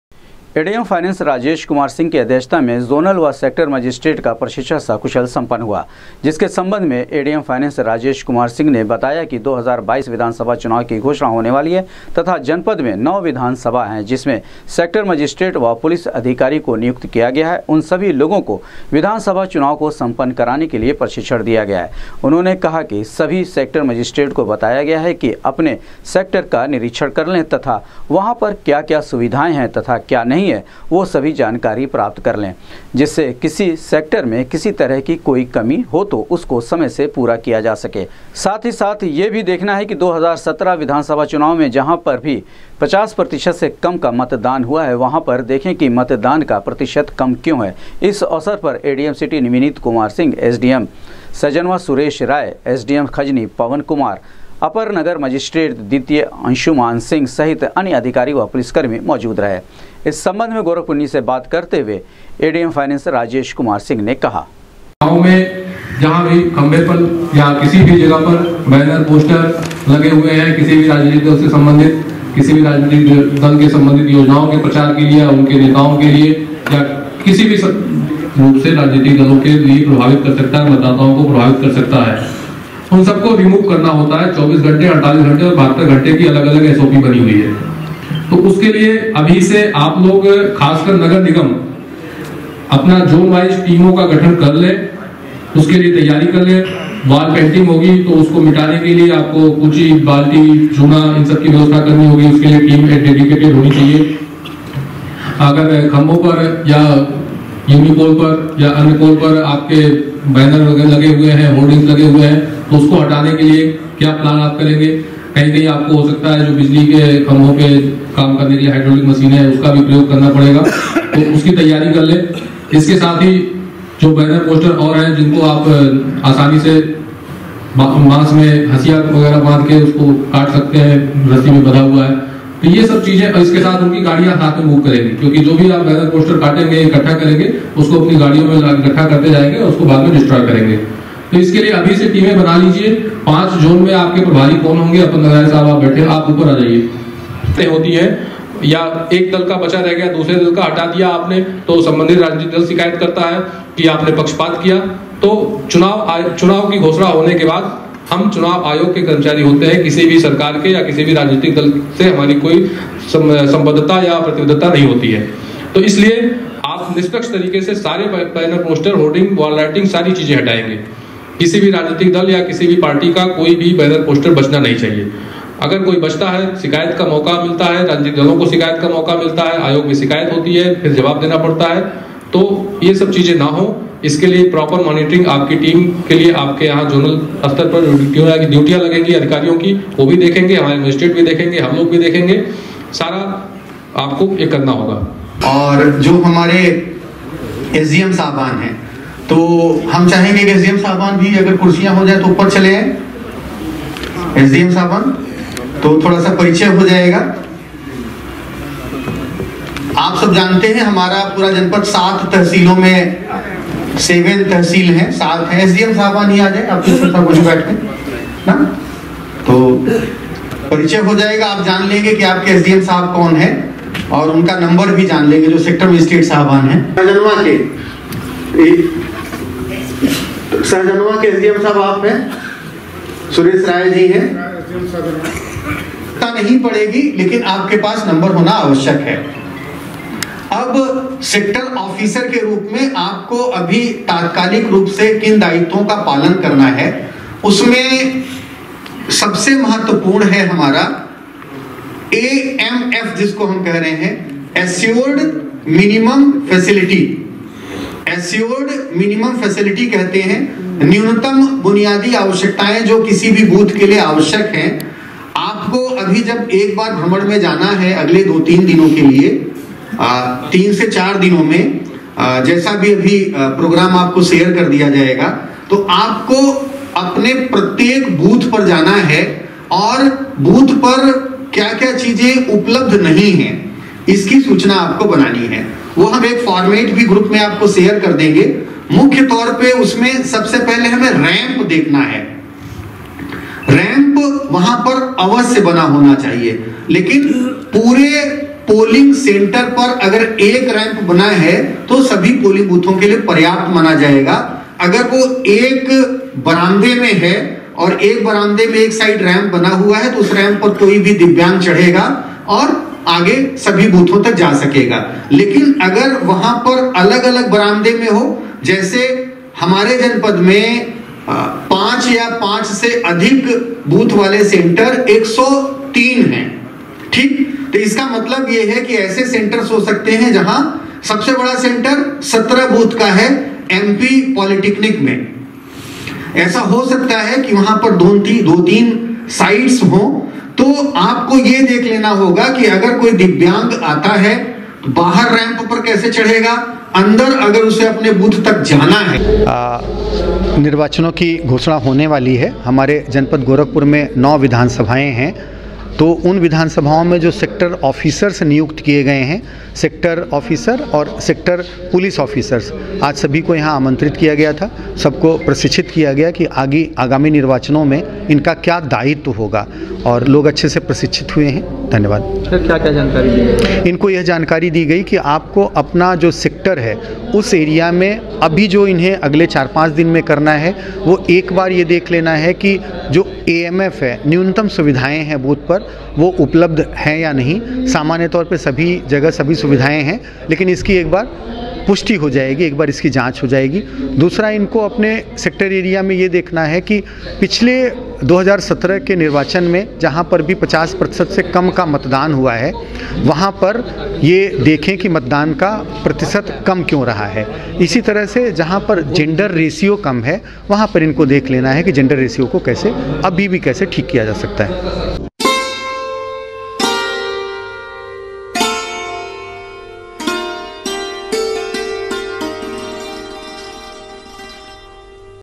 एडीएम फाइनेंस राजेश कुमार सिंह की अध्यक्षता में जोनल व सेक्टर मजिस्ट्रेट का प्रशिक्षण सकुशल हुआ
एडीएम फाइनेंस राजेश कुमार सिंह की अध्यक्षता में जोनल व सेक्टर मजिस्ट्रेट का प्रशिक्षण सकुशल हुआ संपन्न,विधानसभा 2022 चुनाव को संपन्न कराने के लिए दी गई विस्तृत जानकारी